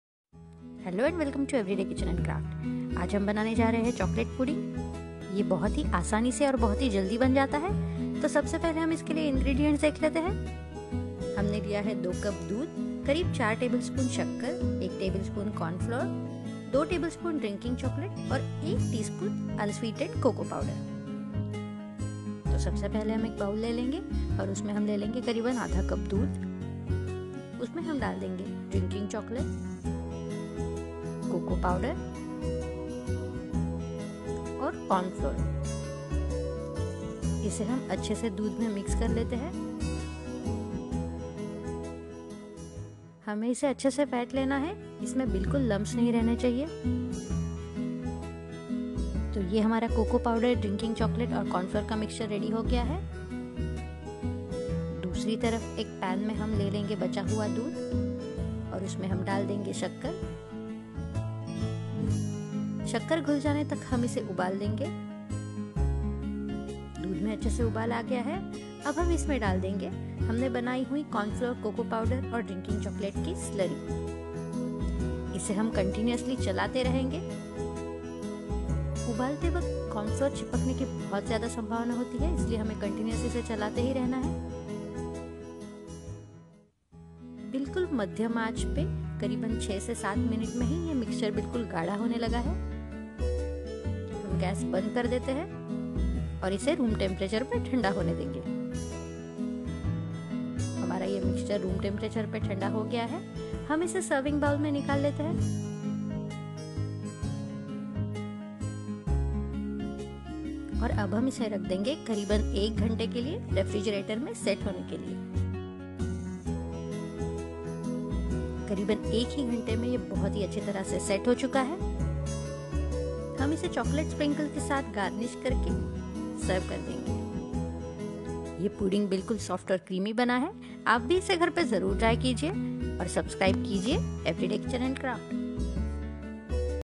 Eggless chocolate pudding |Soft and creamy Chocolate pudding | Chocolate Mousse pudding | no gelatin
Chocolate Pudding is a recipe loved by all. It's soft, creamy and delicious.  This video depicts an easy and quick method to make this yummy chocolate pudding at home. I have made this without using gelatin /agar agar. Do try this dessert recipe at home.